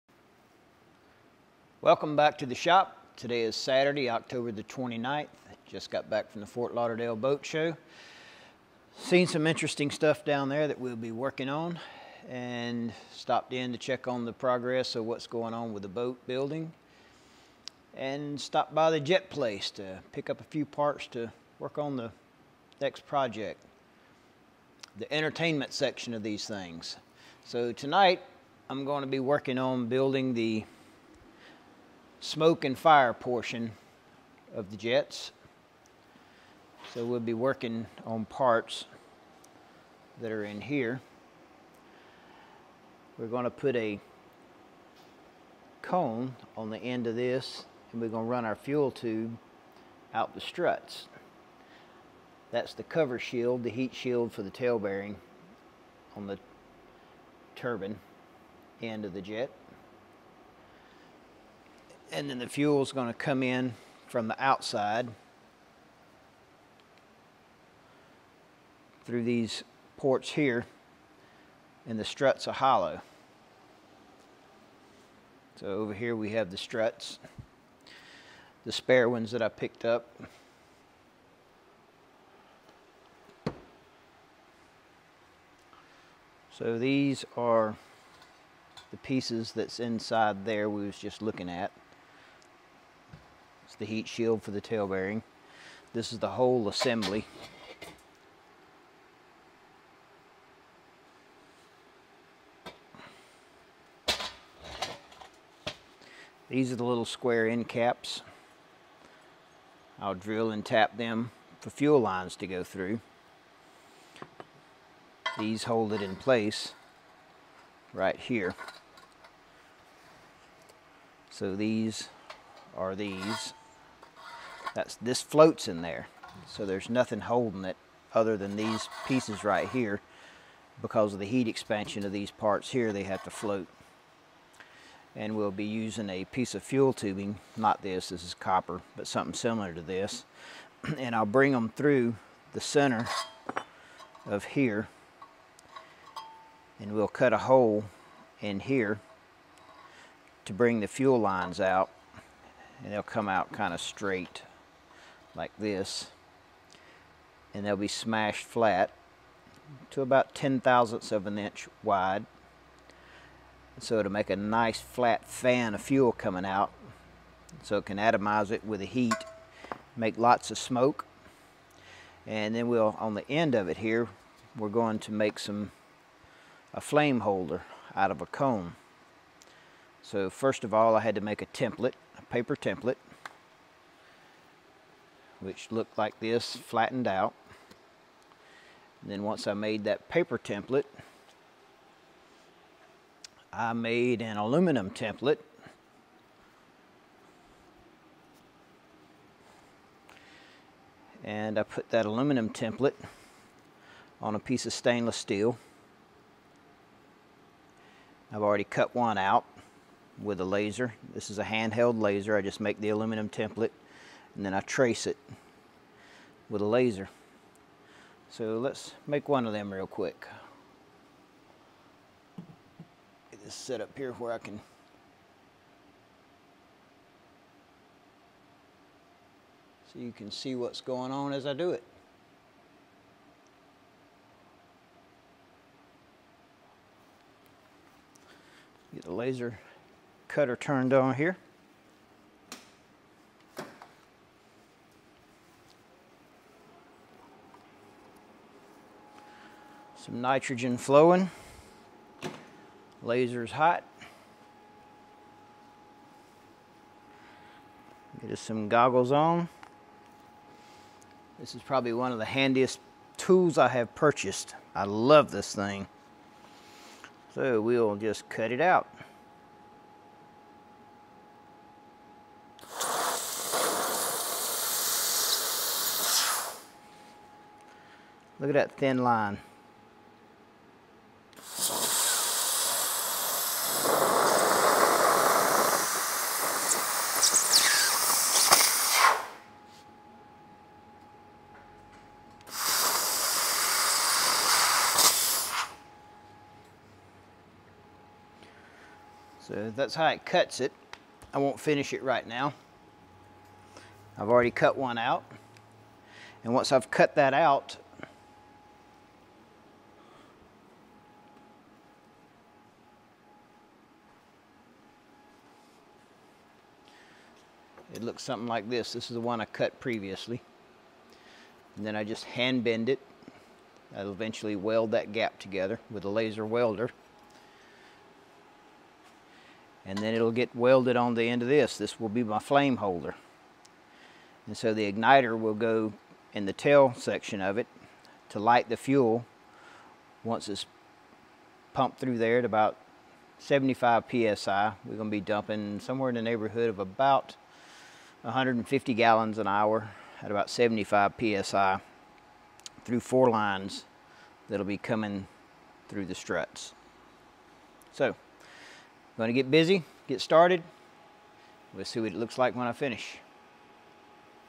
Jet Entertainment, Fire & Smoke!
In this video I’ll be going over the Fire and Smoke parts of the Jet Powered 42’ Cat Project.